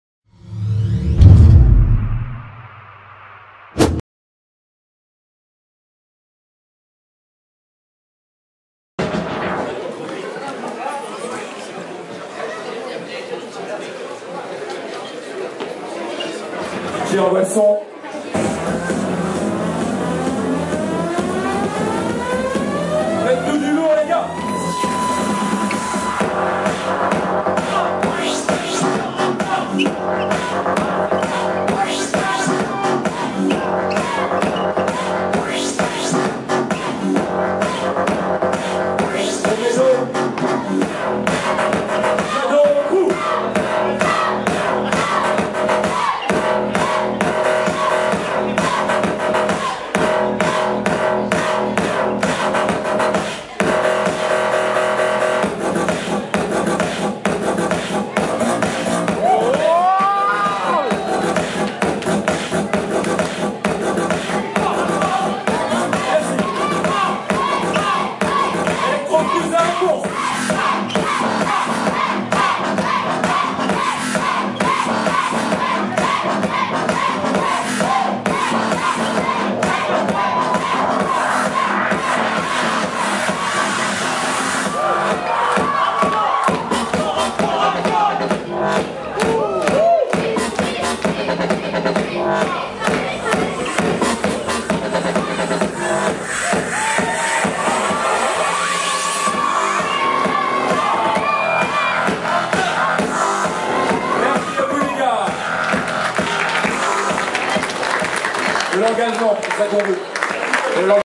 FREEZE VS MEGO | BATTLE 8TH FINAL ELECTRO | URBAN MIX CONTEST 3 (UMC)
Freeze Win